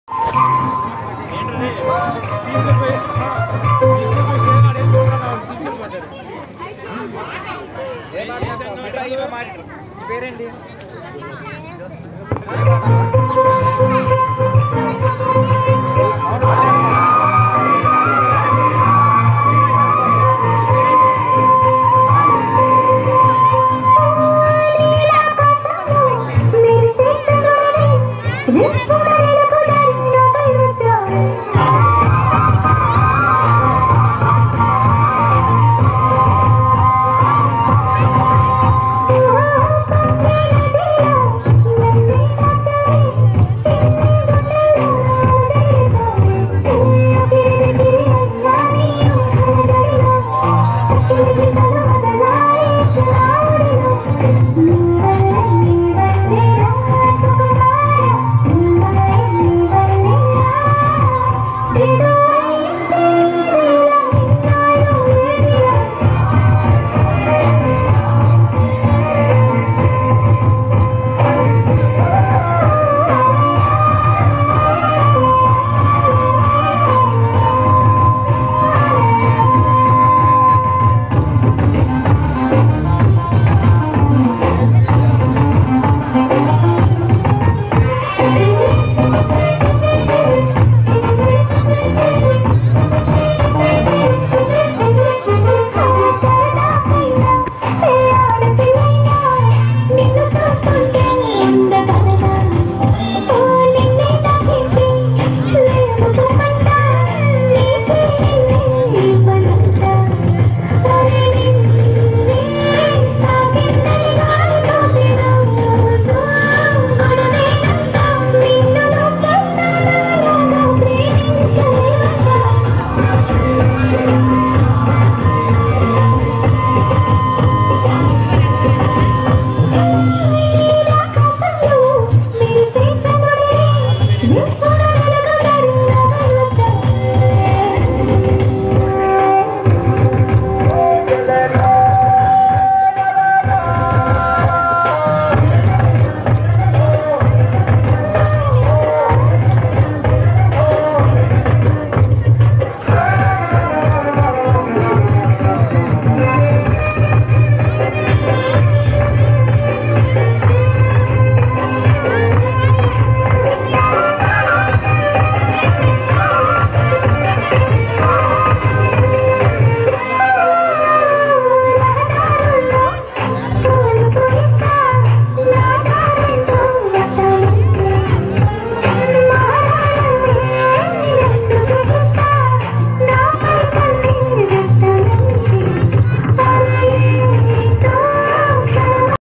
సంక్రాంతి సంబరాల్లో భాగంగా చిన్నారుల డాన్సులు
హెల్పింగ్ హ్యాండ్స్ ఆద్వర్యంలో సంక్రాంతి సంబరాల్లో భాగంగా చిన్నారుల డాన్సులు